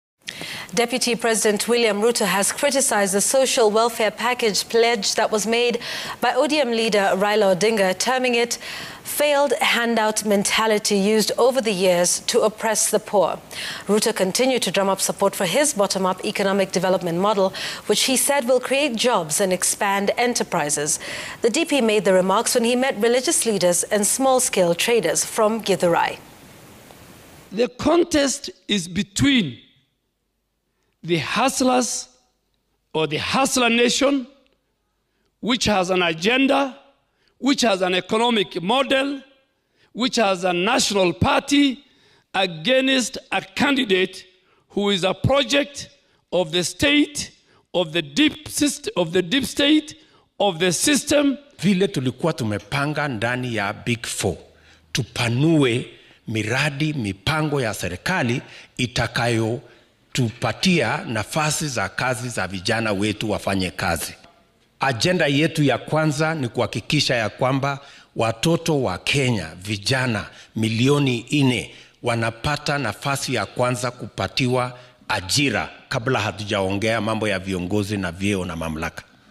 DP William Ruto met a delegation of opinion shapers from Githurai
DP Ruto says 2022 contest between ‘bottom up’ and ‘trickle down’